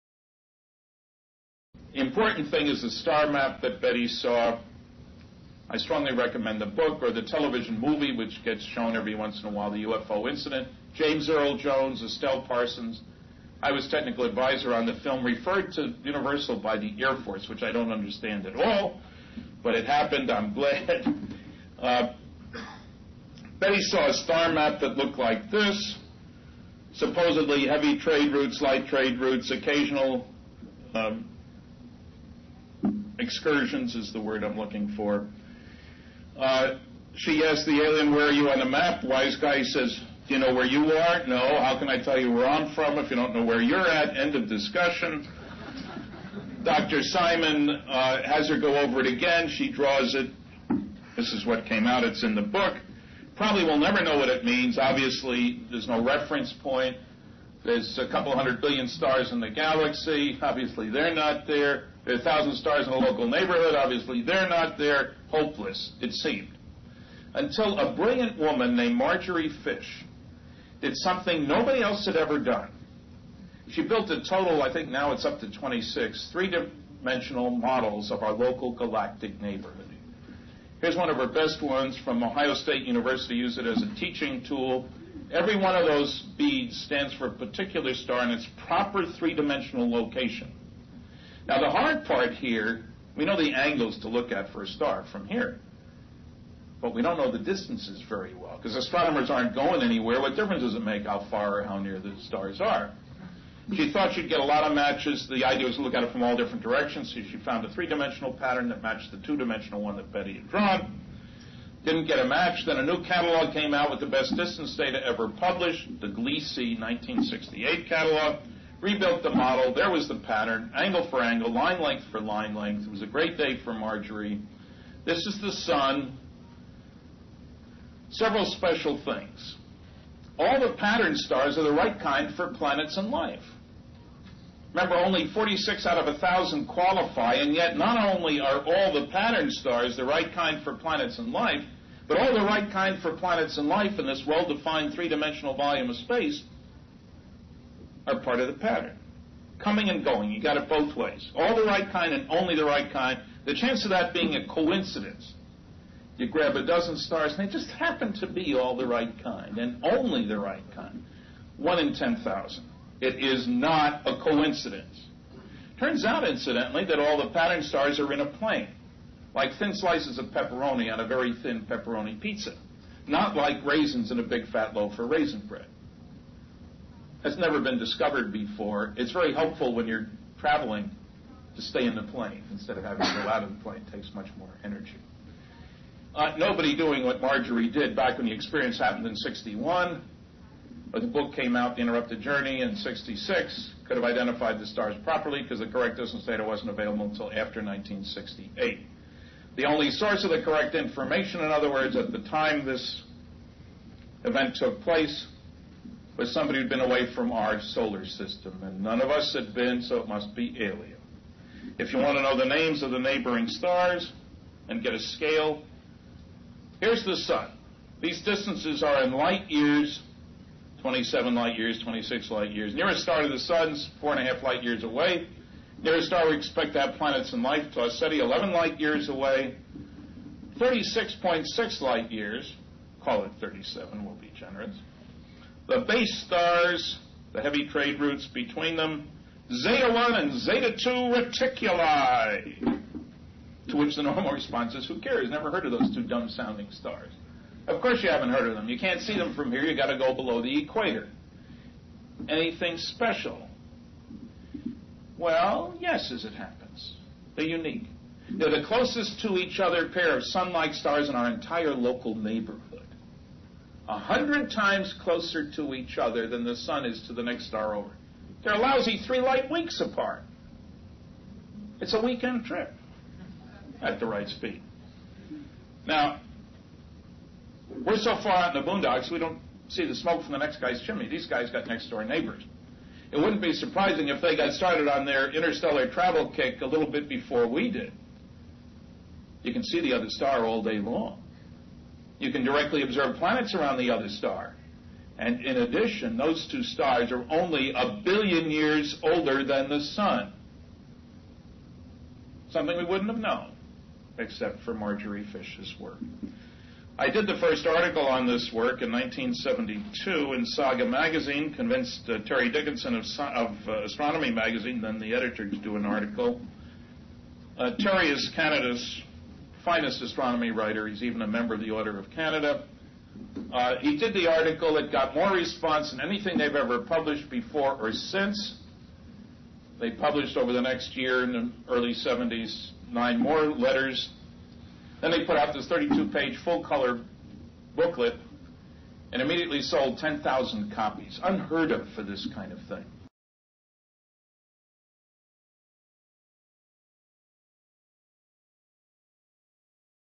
Star Map & Stanton Friedman
Stanton Friedman comments on the validity of the Betty Hill Star
Map. Date unknown